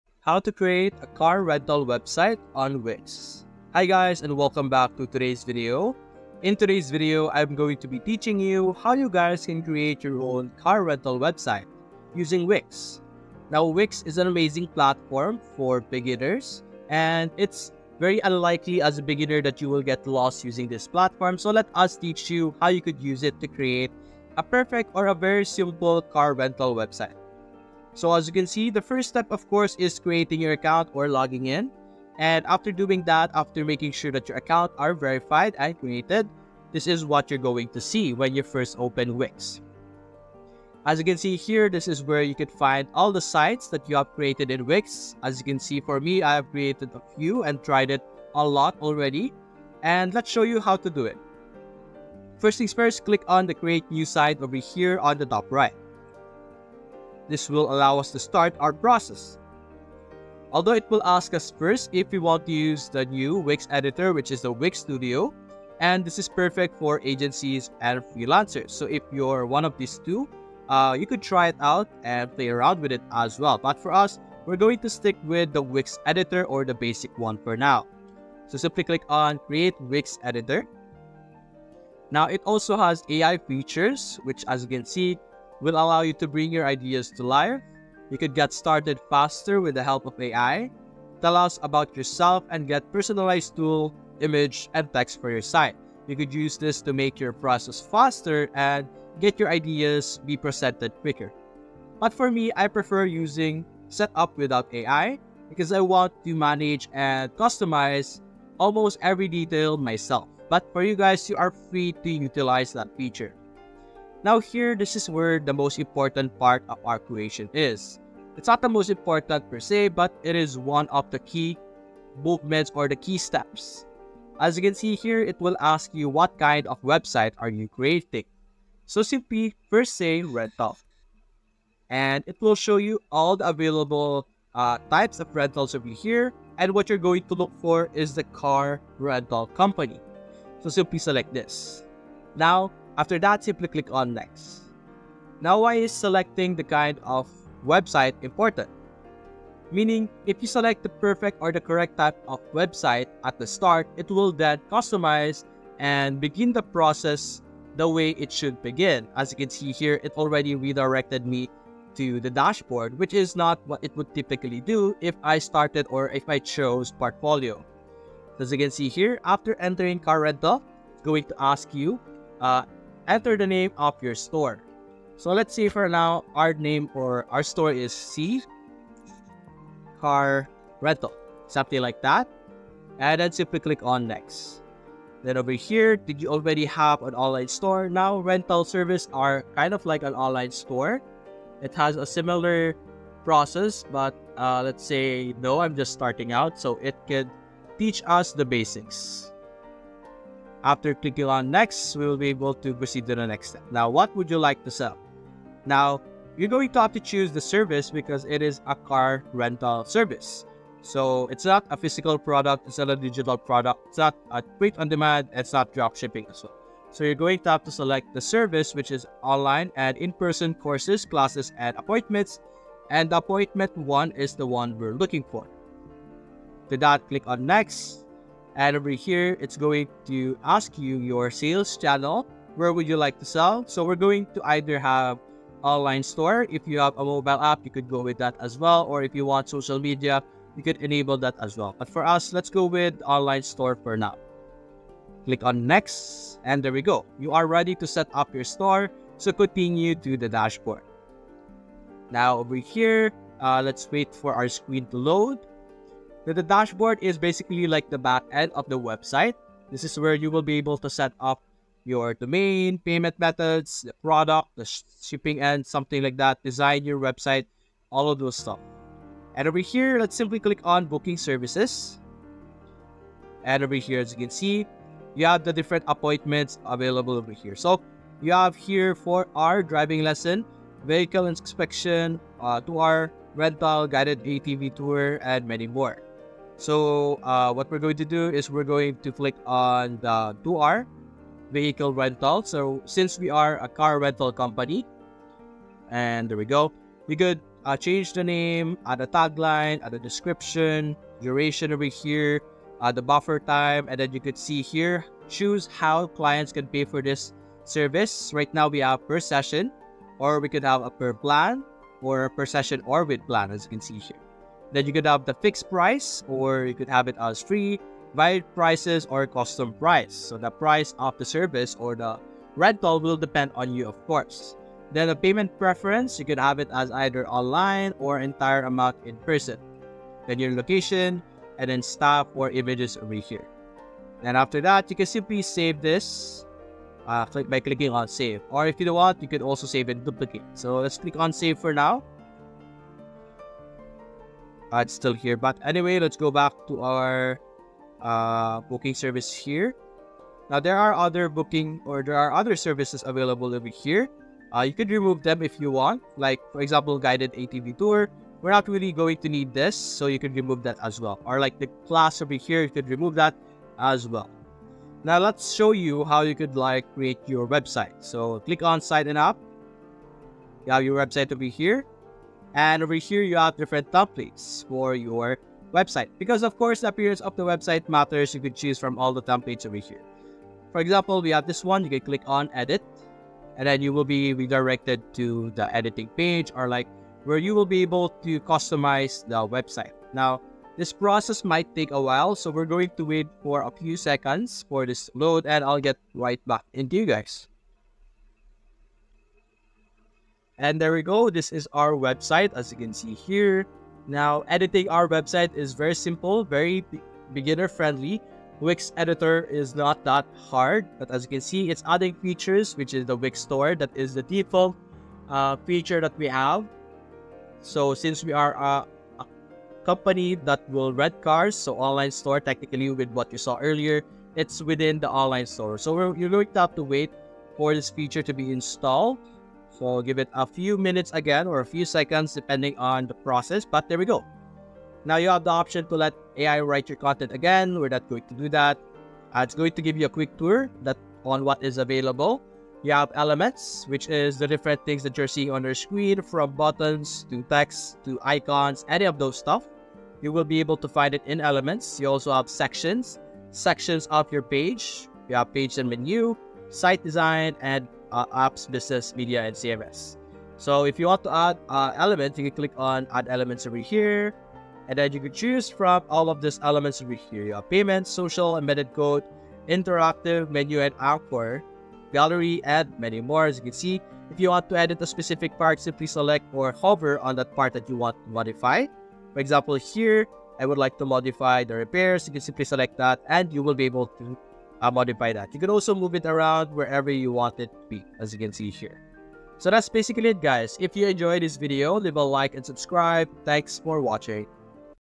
How to Create A Car Rental Website in WIx (Best Method)
How to Create A Car Rental Website in WIx. In this video I'll show you How to Make Car Rental Website in WIx

This is an easy process so keep watching to learn all you need to know about